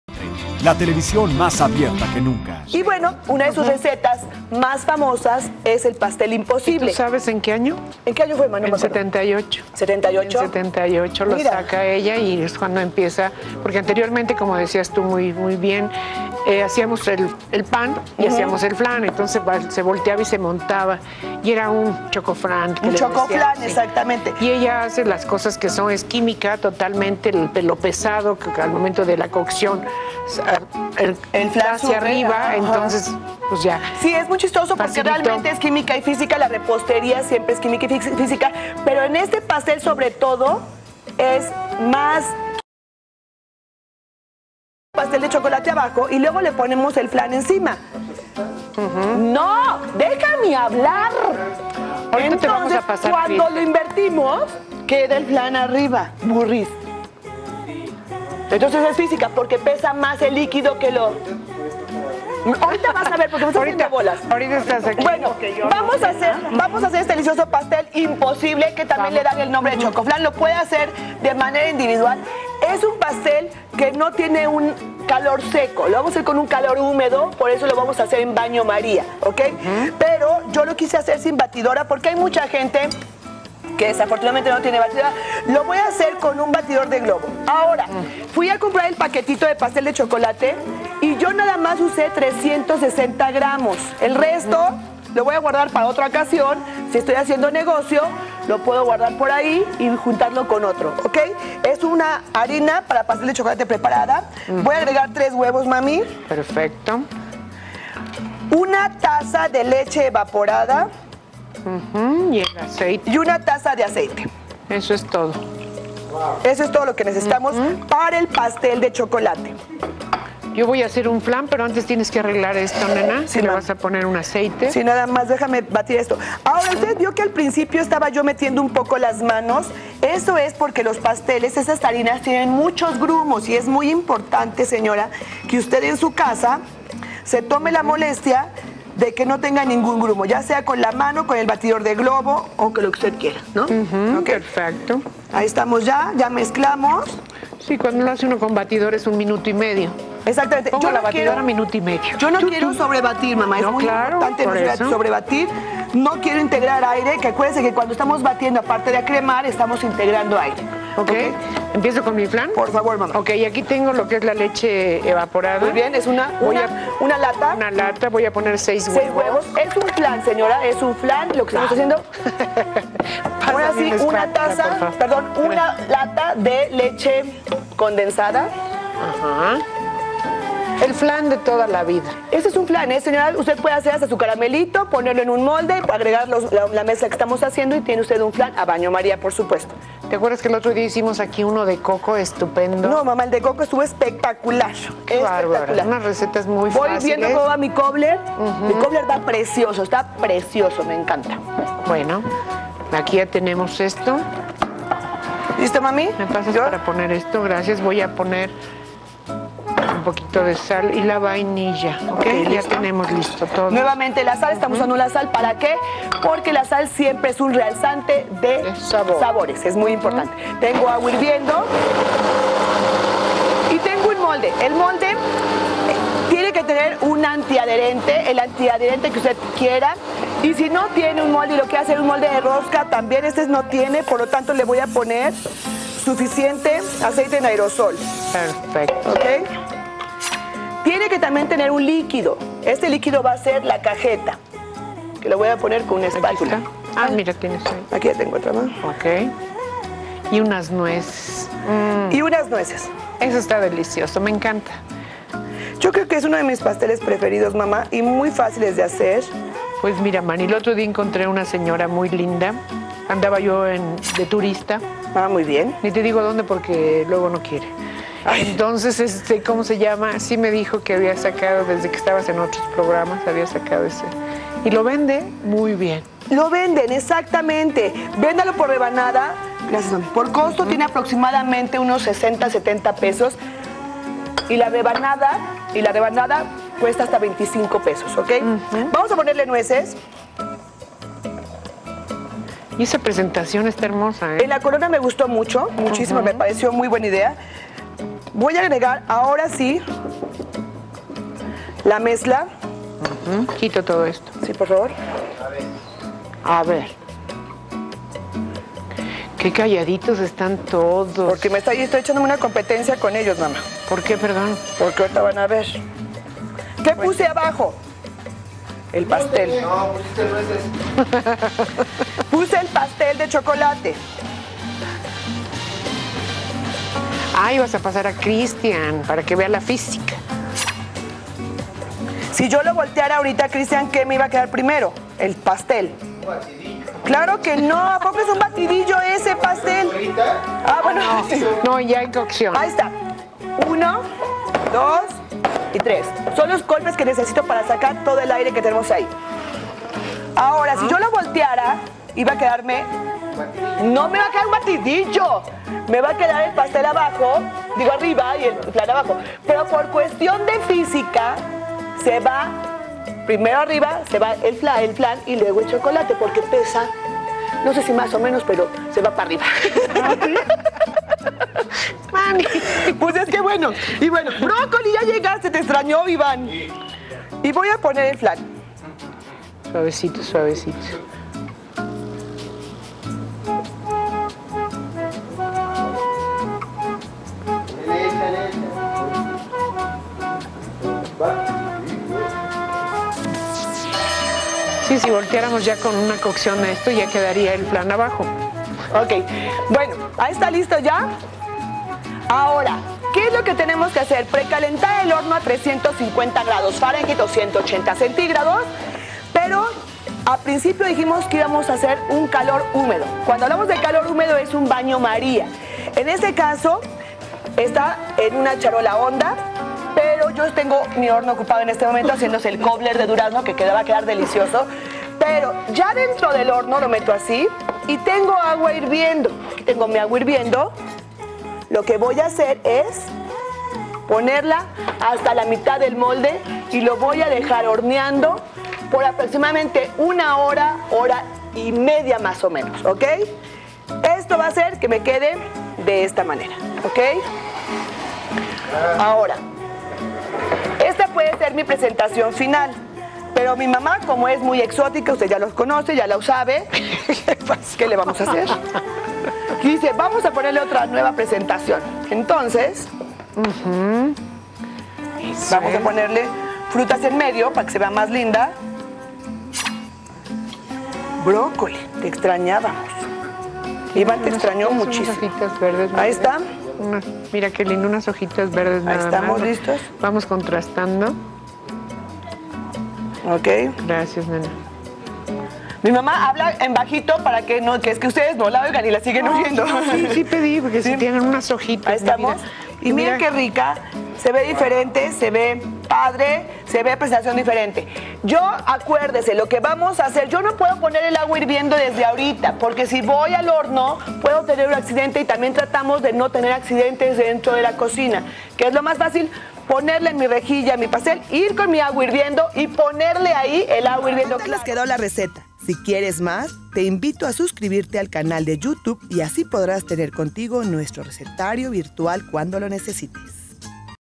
Pastel imposible / Pastel impossible
01 mayo 2014

Ingredientes: Pastel chocolate: cucharadas mantequilla, 1 taza cajeta, 360 gr harina para pastel de chocolate, 3 huevos, 1 taza aceite, 1 taza leche evaporada, 1/2 taza nuez picada. Flan: 1 lata leche condensada, 1 lata de leche evaporada, 6 huevos, 1 cucharada vainilla.